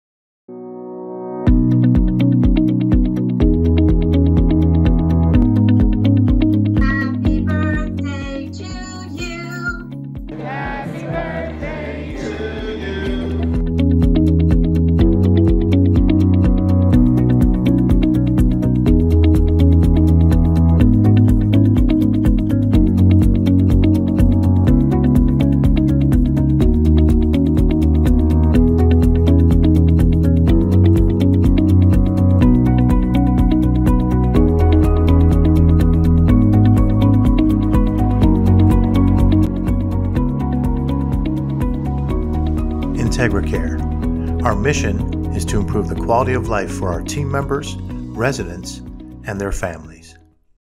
Resident Engagement During the Covid-19 Crisis
Many family members who are unable to visit residents turned to the virtual hugs to share messages with their loved ones or team members. Through mid-April, 329 virtual hugs and 178 virtual visits had been made via a portal on the IntegraCare website.